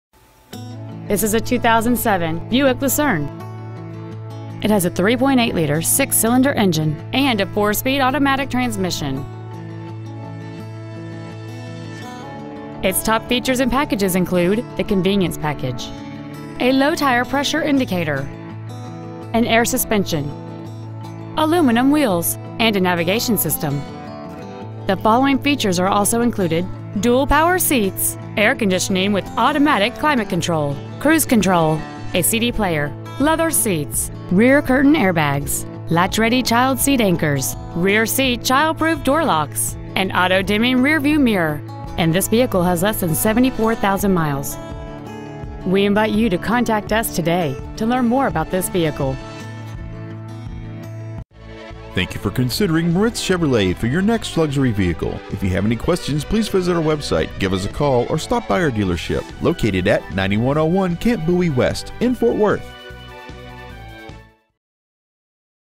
2007 Buick Lucerne Waco TX
We have been honored to serve the Fort Worth TX area , we promise that your experience at our dealership will exceed your expectations ! Year : 2007 Make : Buick Model : Lucerne Engine : V6 3.8L Trans . : Automatic Exterior : White Opal Miles : 73,948 Stock : V126219A Moritz Chevrolet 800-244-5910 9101 Camp Bowie West Fort Worth , TX 76116